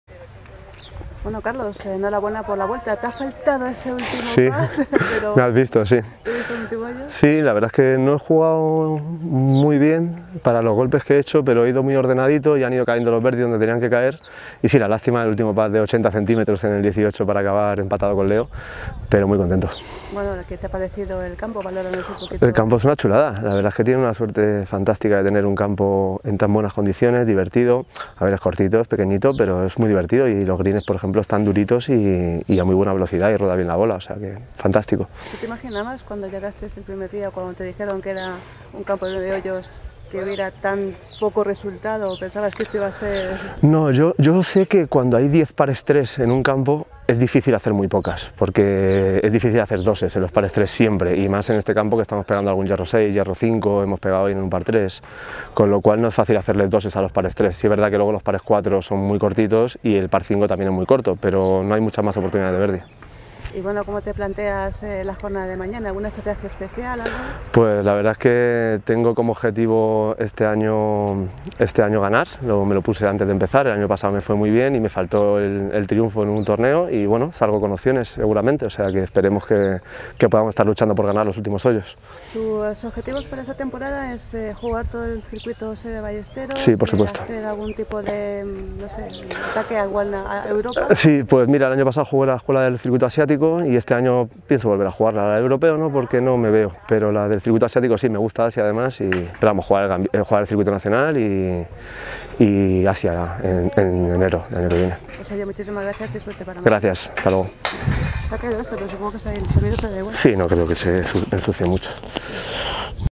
Entrevista a Carlos Sánchez
Entrevista a Carlos Sánchez, tercero con -9 tras la primera ronda del Torneo de Golf de Ciudad Real, primera prueba del Seve Ballesteros PGA  Tour 2018